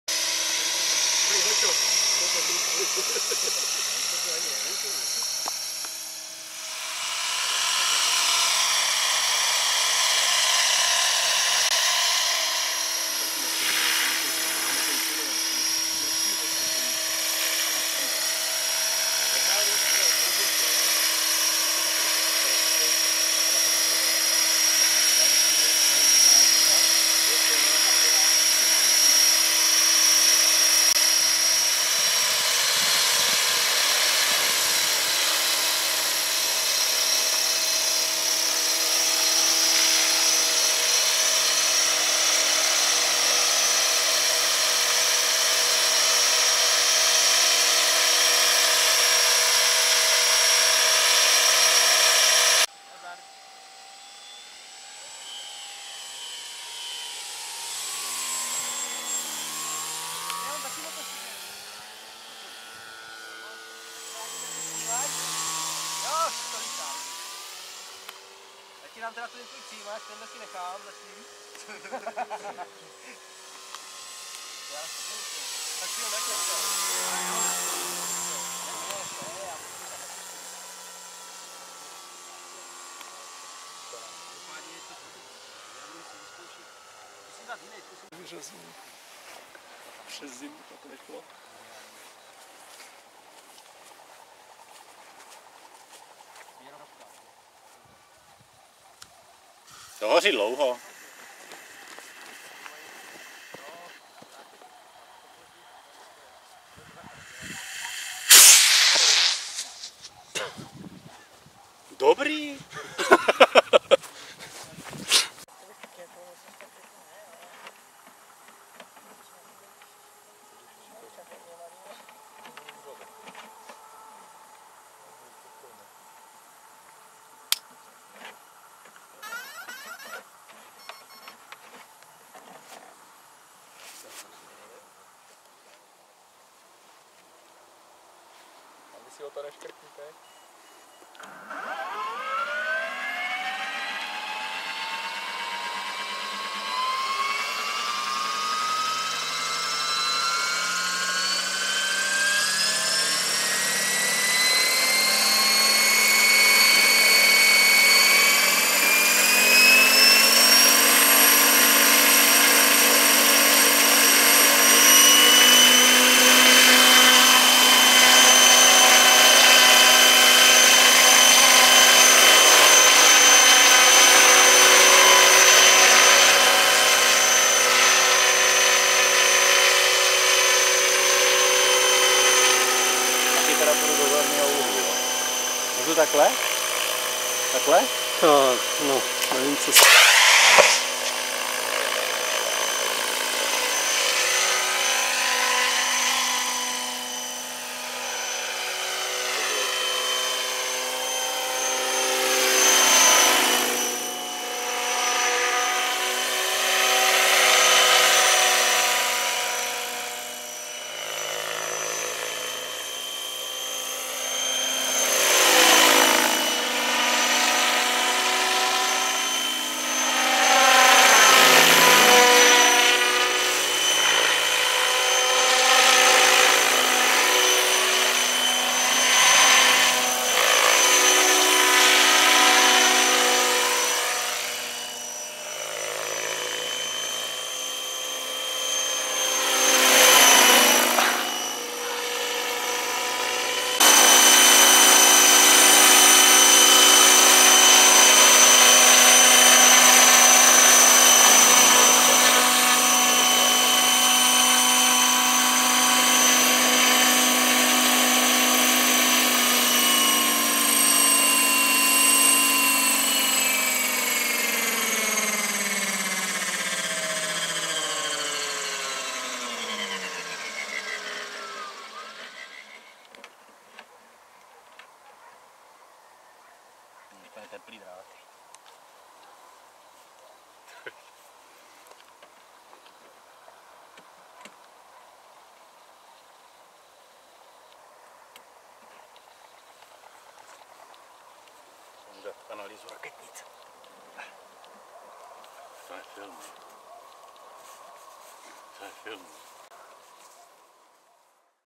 Hughes 500MD, T-Rex
Na letišti u Rovenska létá Pavel, Jirka a Lukáš